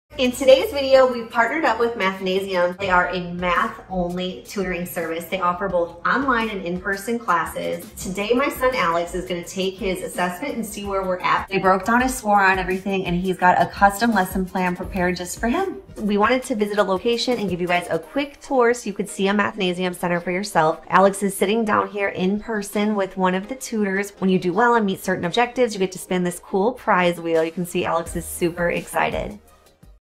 Social Media Influencer Family Testimonial, Young Wild Me
The social media influencer family, Young Wild Me, speaks about their experience with Mathnasium.
Runtime: 30 Seconds